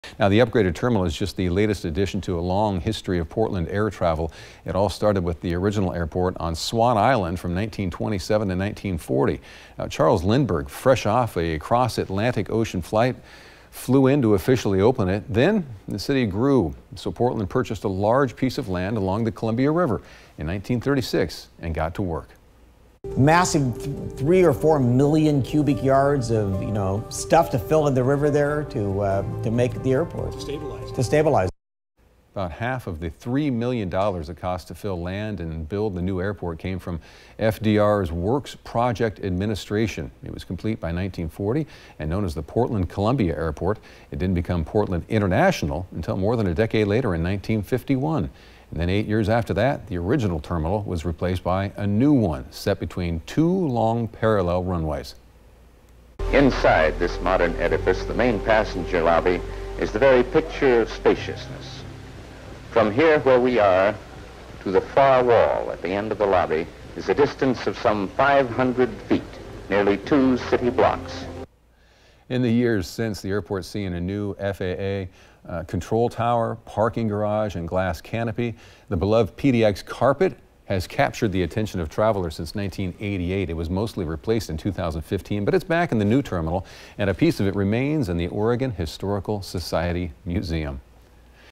How the Portland Airport became PDX
A new terminal is just part of the decades-long history of the Portland International Airport.